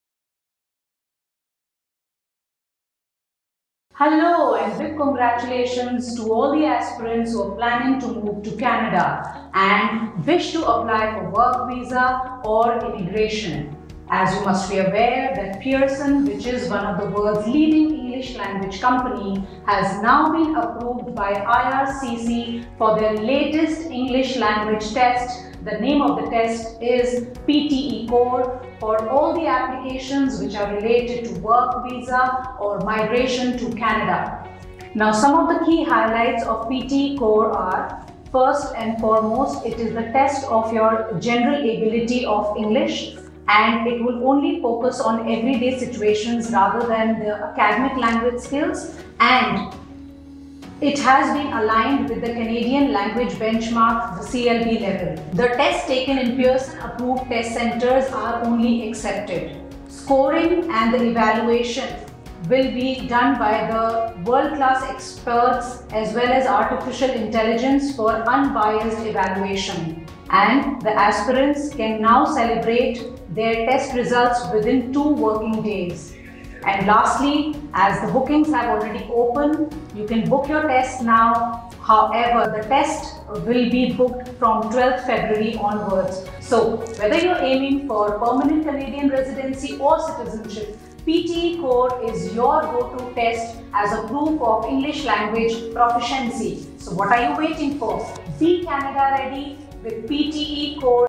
PTE CORE Valid For CANADA | New IRCC Update| Canada Immigration Updates 2024| Study in Abroad 2024
00:00:03 Introduction
00:00:22 PTE Core Test Overview
00:00:34 Key Highlights of PTE Core
00:01:00 Test Scoring and Bookings